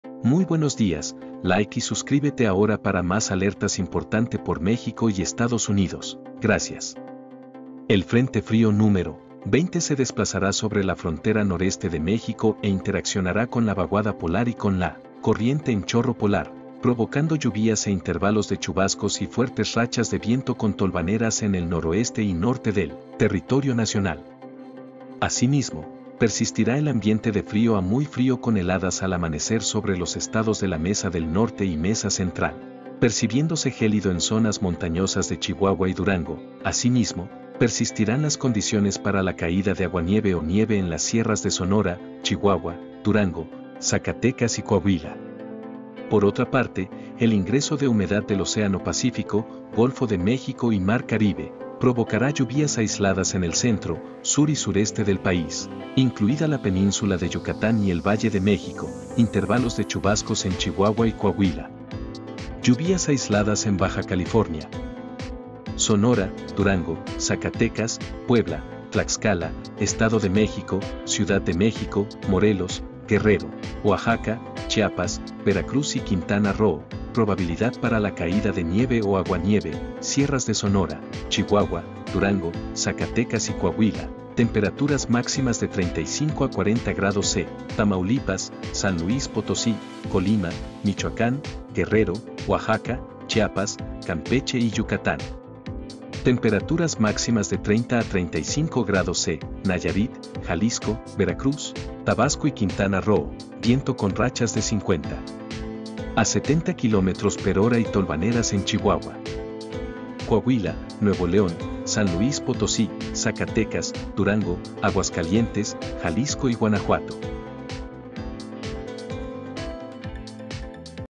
🟡 AVISO ESPECIAL 🟡 FRENTE FRÍO 20 INTERACTUANDO CON VANAL POLAR
Pronóstico De Lluvias Intensas Para México y Frente Frío Número 20 y Vientos Info Durante 30 de Diciembre de 2022.